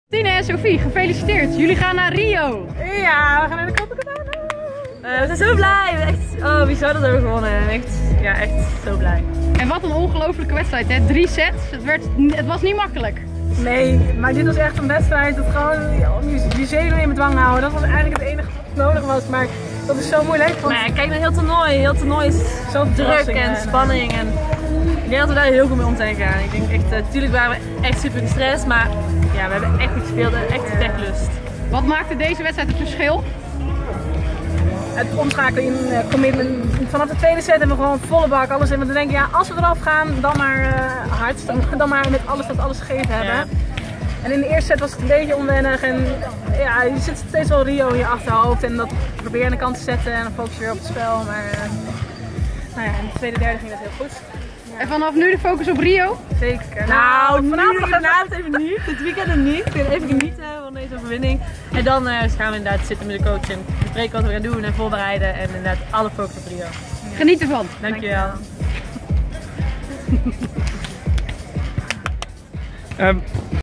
Interview Sophie van Gestel & Jantine van der Vlist na kwalificatie voor Olympische Spelen 2016
Nederland won zaterdag 25 juni 2016 in het Noorse Stavanger de CEV Beach Volleyball Continental Cup. Sophie van Gestel / Jantine van der Vlist en Rimke Braakman / Jolien Sinnema wonnen goud door Oekraïne in de finale met 2-1 te verslaan. Door deze fantastische prestatie mag Beachvolleybal Team Van der Vlist/Van Gestel deelnemen aan de Olympische Spelen van Rio de Janeiro. Bekijk hier hun reactie na afloop!